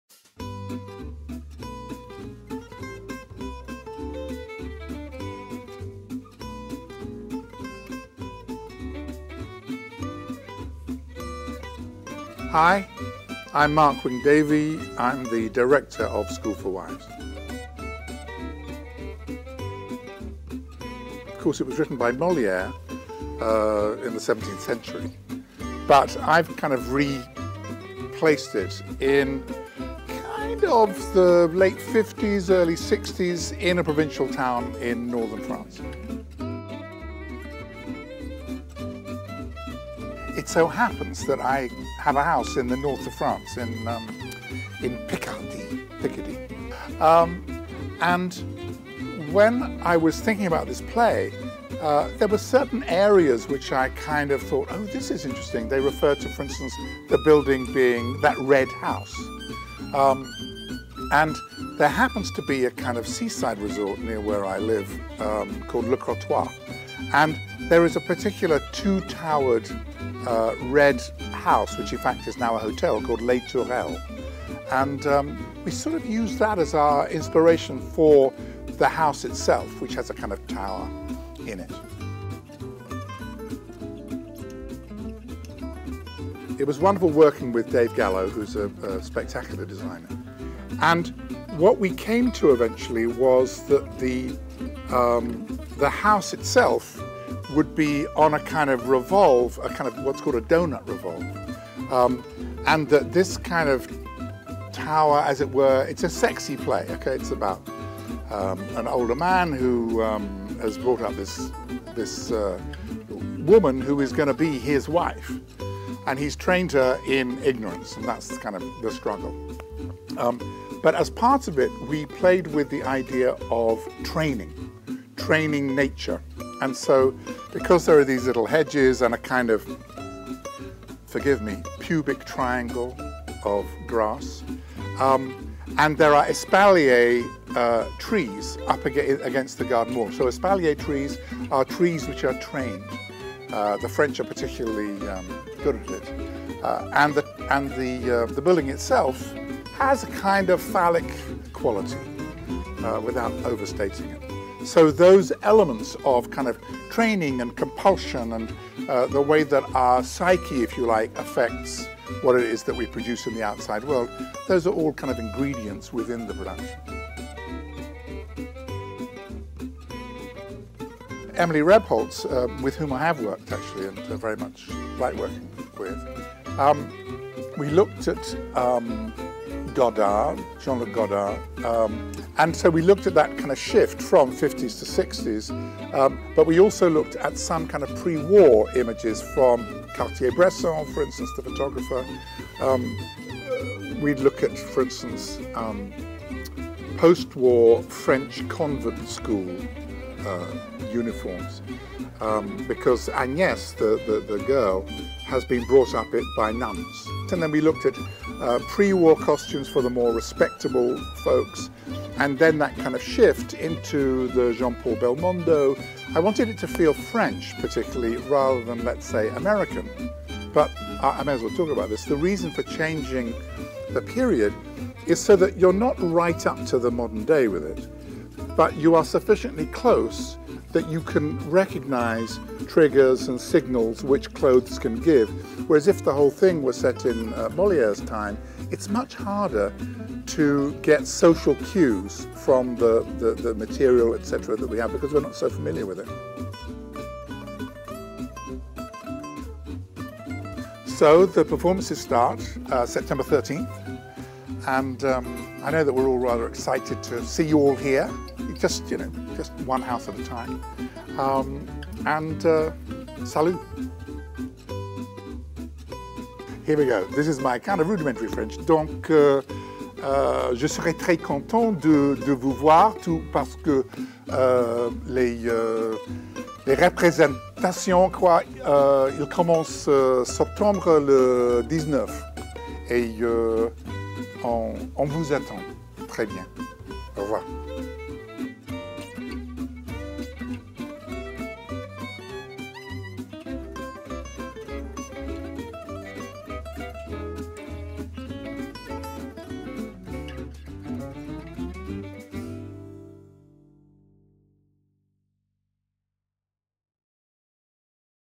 Interview with Mark Wing-Davey, Director of "THE SCHOOL FOR WIVES"
Interview with Mark Wing-Davey, director of our 2014 production of "The School for Wives", written by Moliere. Wing-Davey takes us into the inspiration of the look of this production and what kinds of research went into creating this world.

The School For Wives
September 13, 2014 - October 5, 2014
By Molière
Translated into English Verse by Richard Wilbur
Directed by Mark Wing-Davey

Two River Theater, Red Bank, NJ

We open our season with Molière’s classic comedy about a rich, middle-aged man named Arnolphe, whose plan to groom his innocent young ward into the “perfect” wife goes quickly awry. Can this pompous bachelor really be a match for rival suitors, inept servants, and the wit of his soon-to-be bride? This true classic, written by the master of French farce, is not to be missed!

Video by Mike Boylan | Pennant Collective